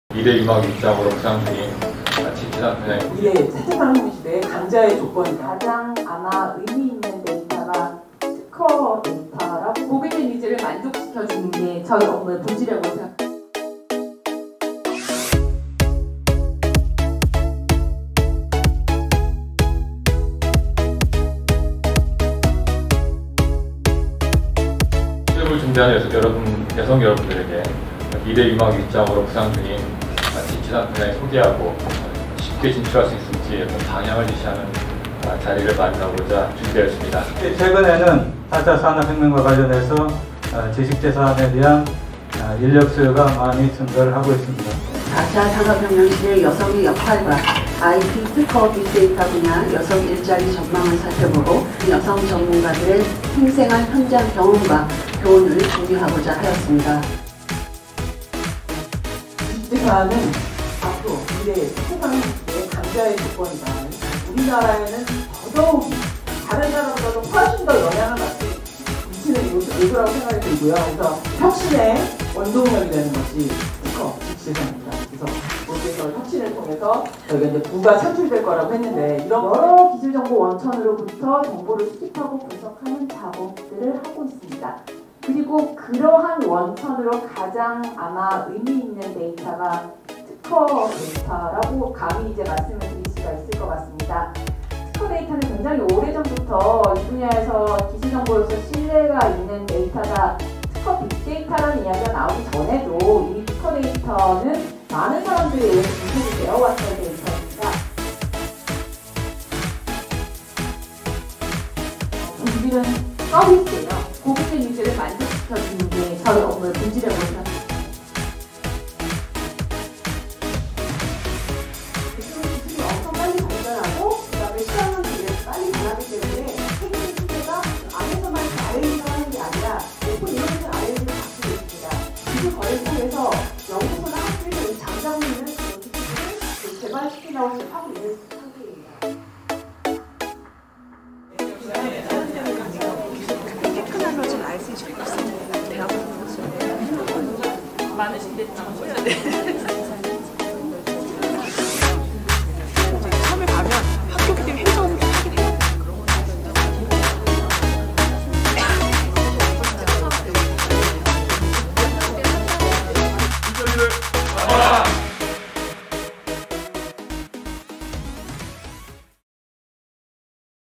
[WISET] WOMEN@PATENT : 지식재산의 미래와 여성
지난 9월 6일, 한국과학기술회관 12층 아나이스홀에서는 ‘WOMEN@PATENT : 지식재산의 미래와 여성’이라는 주제로 4차 산업혁명시대의 유망 직종인 ‘지식재산(IP)’ 분야의 가능성과 여성 일자리 전망을 알아보는 컨퍼런스가 개최되었습니다.

 다양한 특허분야 전문가의 기조연설과 현직 선배의 이야기를 들을 수 있는 TED토크, 그리고 연사분들이 직접 함께한 일자리 멘토링까지! 다양한 프로그램으로 정말 많은 분들을 만날 수 있는 시간이었죠!